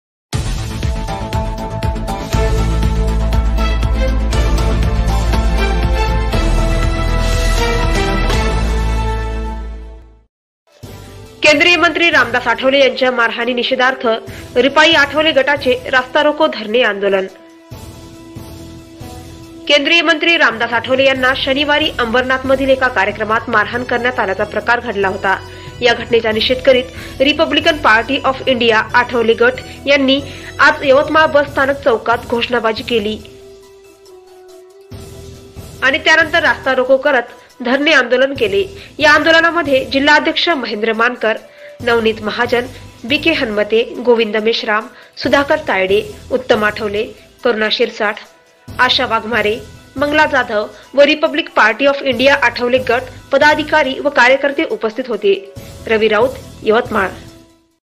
रिपाई आठवले गटाचे रास्ता रोको धरणे आंदोलन....
रवी राऊत/यवतमाळ

केंद्रीय मंत्री रामदास आठवले यांना शनिवारी अंबरनाथमधील एका कार्यक्रमात मारहाण करण्यात आल्याचा प्रकार घडला होता या घटनेचा निषेध करीत रीपब्लिकन पार्टी ऑफ इंडिया आठवले( गट)यानी आज यवतमाळ बस स्थानक चौकात घोषणाबाजी केली आणि त्यानंतर रास्ता रोको करत धरने आंदोलन केले.